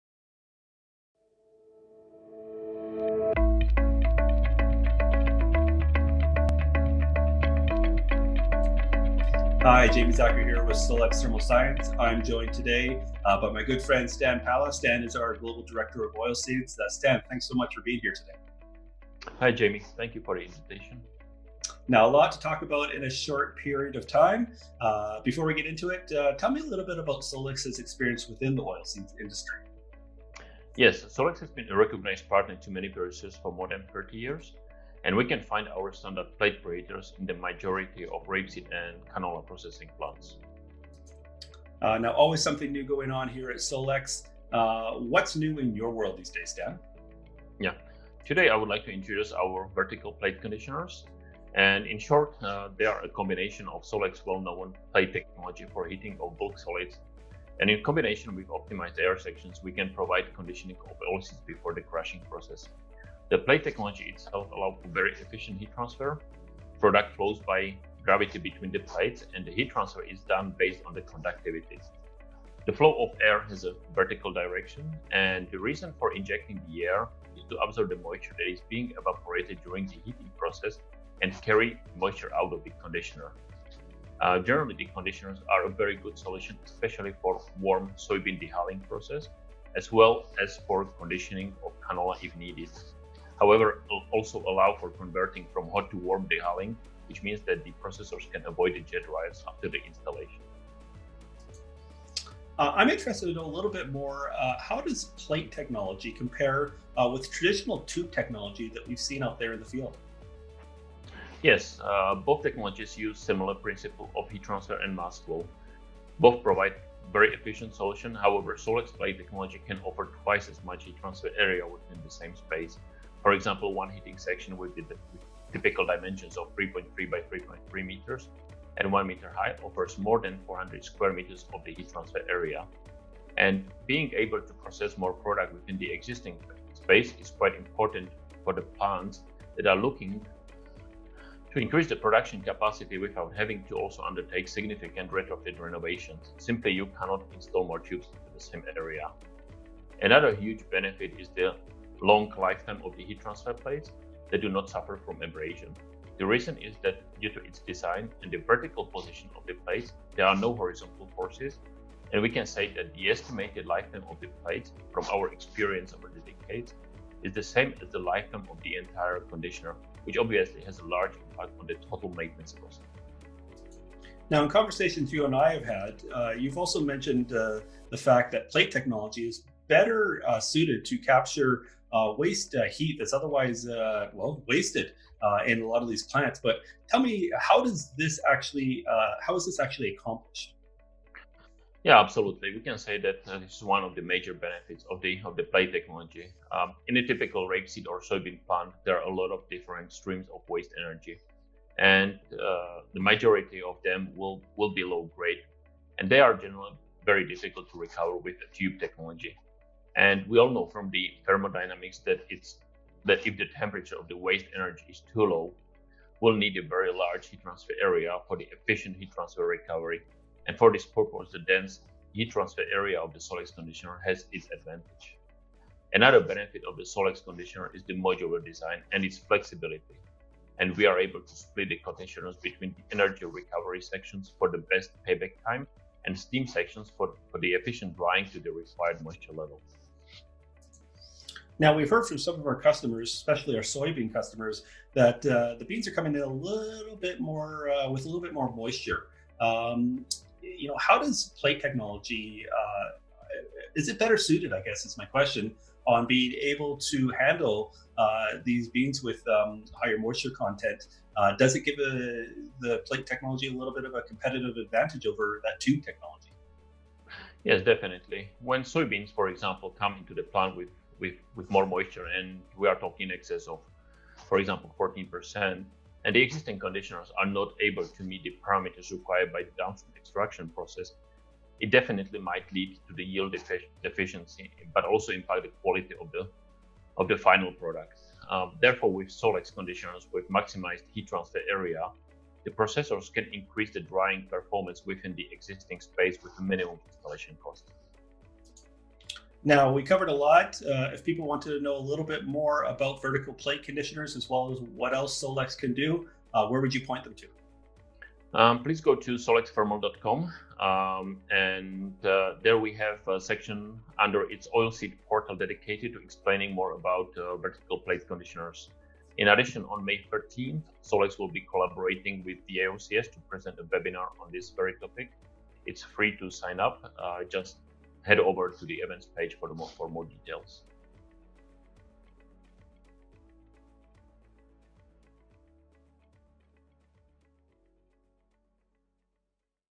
Vertical Plate Conditioners for Oilseeds Processing
Stan Pala, Global Director, Oilseeds, sits down to discuss how Vertical Plate Conditioners (VPCs) provide the oilseeds industry with a modular solution that allows for efficient heat transfer in a well-established welded plate-channel design.